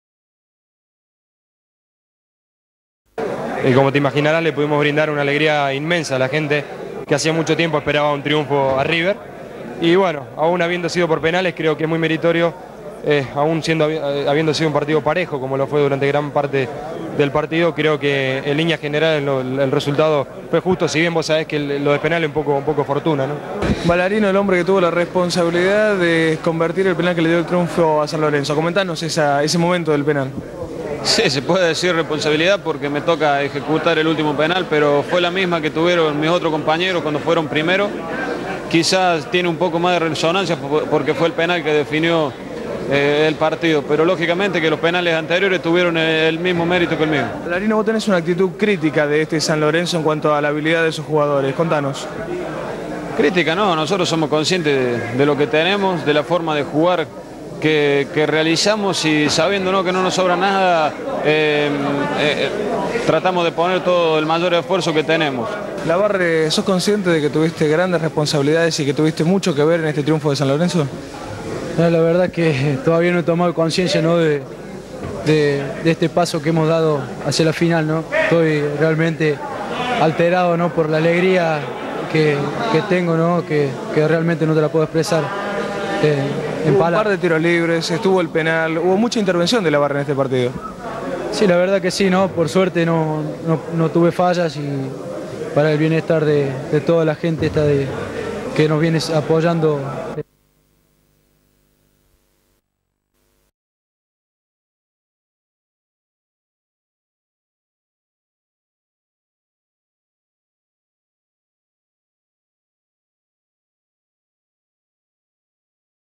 Fabian Carrizo - Mario Ballarino - Cesar Labarre - Derrota River Plate 1991
Bs. As.: Reportaje en los vestuarios al futbolista Fabian Carrizo (San Lorenzo de Almagro) donde habla del triunfo por penales contra River Plate en la semifinal de la liguilla Pre Libertadores. Insert secuencias breves del partido jugado en el estadio de Huracan. Imagen de la tribuna con los hinchas. Reportaje a Mario Ballarino: -¿Sos el hombre que tuvo la responsabilidad de convertir el penal que le dio el triunfo a San Lorenzo, contanos ese momento del penal? -¿Vos tenes una actitud crítica de este San Lorenzo en cuanto a la habilidad de sus jugadores, contanos? Reportaje a Cesar Labarre: -¿Sos consciente de que tuviste grandes responsabilidades y que tuviste mucho que ver en este triunfo de San Lorenzo? -¿Hubo un par de tiros libres, estuvo el penal, hubo mucha intervención de Labarre en este partido?
(Partido de vuelta semifinal del torneo octogonal 1991)
Arbitro: Juan Carlos Crespi
Referencia: En la definición por penales, ganó San Lorenzo 4 a 1. Para San Lorenzo convirtieron: Carlos Bustos, Fabián Carrizo, Adrián Czornomaz y Mario Ballarino. Para River Plate convirtió: Jorge Gordillo. Leonardo Astrada remató desviado y César Labarre le contuvo el disparo a Juan Sánchez.
Formación de San Lorenzo: Cesar Orlando Labarre, Carlos Bustos, Daniel Riquelme, Flavio Zandona, Fabian Gustavo Carrizo, Mario Ballarino, Victor Hugo Ferreyra, Gustavo Matosas, Adrian Czornomaz, Jose Fabian Albornoz, Roberto Oscar Garcia. DT: Fernando Arean.
Formación de River Plate: Angel David Comizzo, Jorge Higuain, Carlos Enrique, Jorge Gordillo, Leonardo Ruben Astrada, Juan Amador Sanchez, Sergio Berti, Gustavo Zapata, Walter Silvani, Juan Jose Borrelli, Jorge Vazquez. DT: Daniel Passarella.
Fecha: 4/8/1991
Duración: 1 minuto 40 segundos
Código: UG-2739